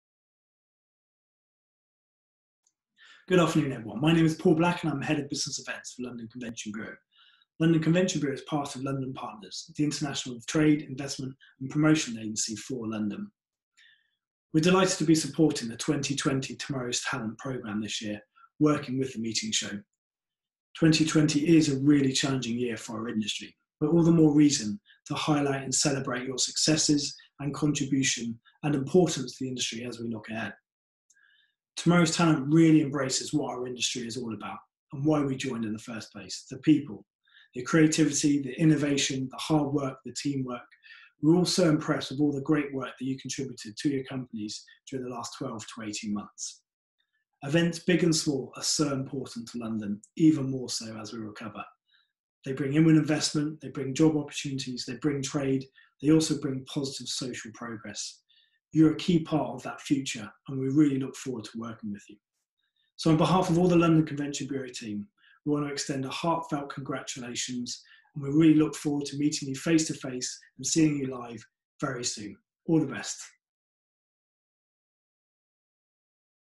Paul Black: Tomorrow's Talent 2020
Ahead of the announcement of our Tomorrow’s Talent 2020 winners on Monday 10 August, sponsor London Convention Bureau's head of business events, Paul Black, shares his thoughts on the importance of celebrating new talent in the meetings and events industry at such a crucial time for the sector.